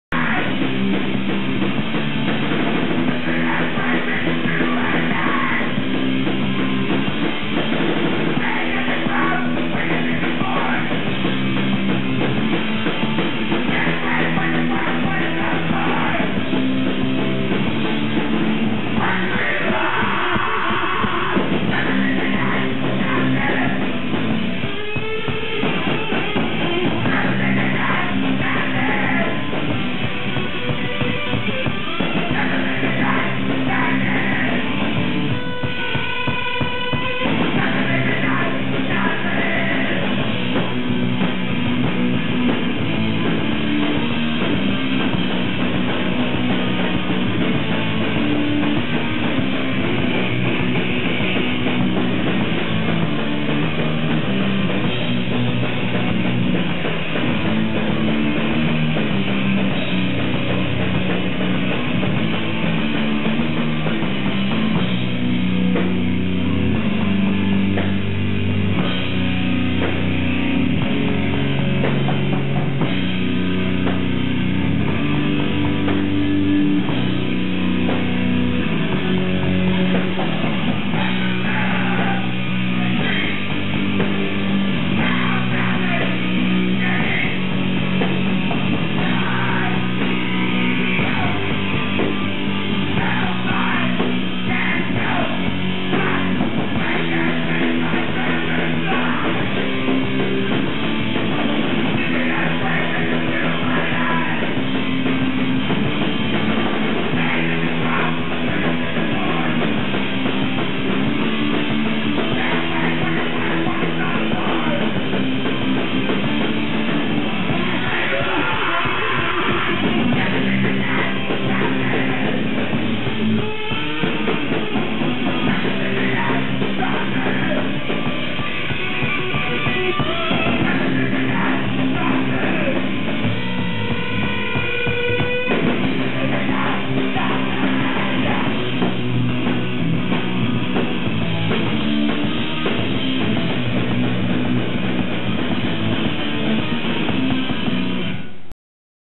Deface 6/28/09
Deface live at the Gilman, Berkeley, CA Check this band out